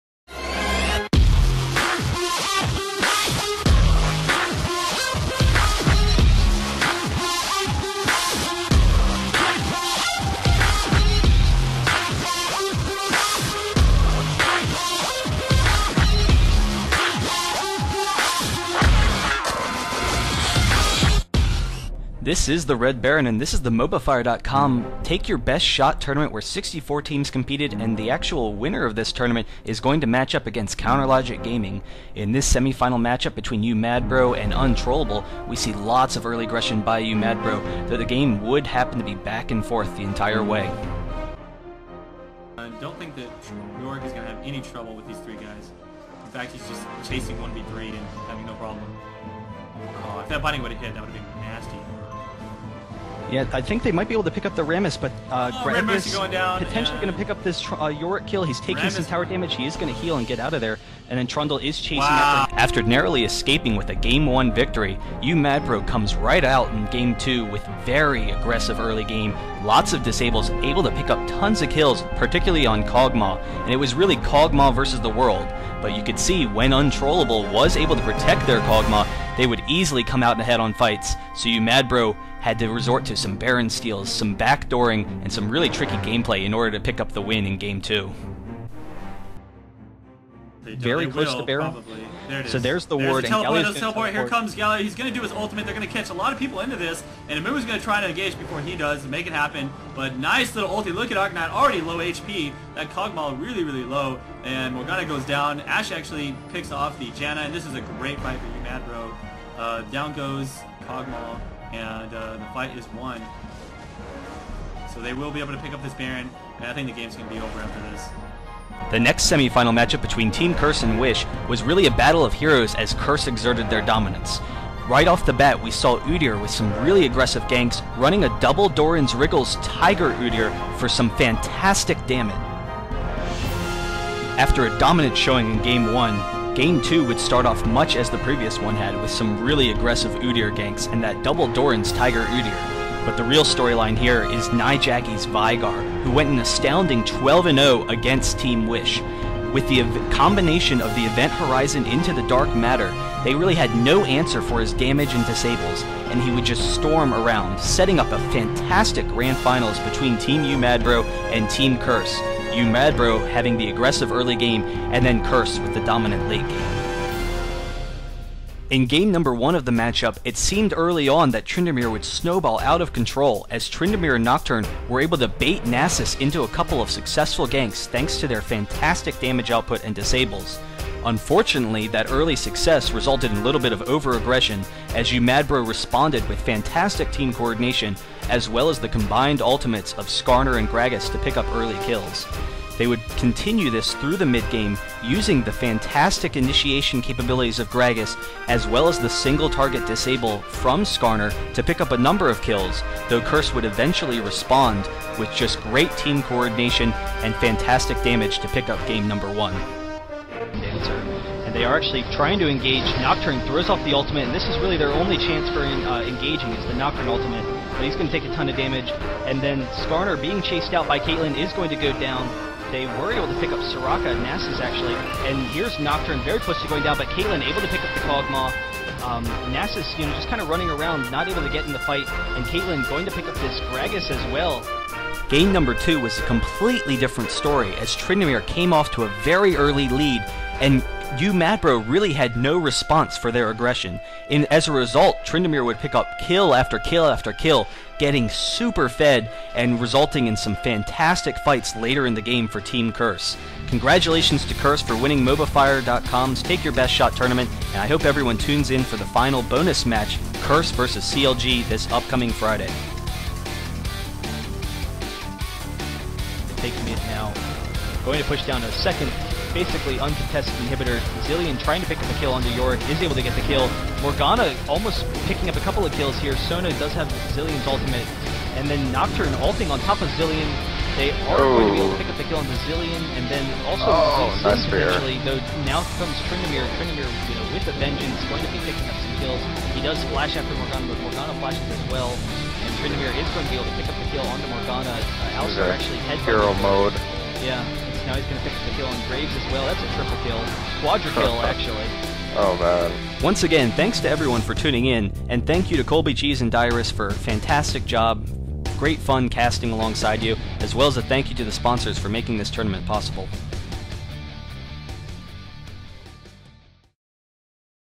MobaFire's "Take Your Best Shot" Tournament
Recap of the MobaFire.com "Take Your Best Shot" Tournament hosted by MobaCast.com.

Thanks to:
Shinsoo - Assistant Editor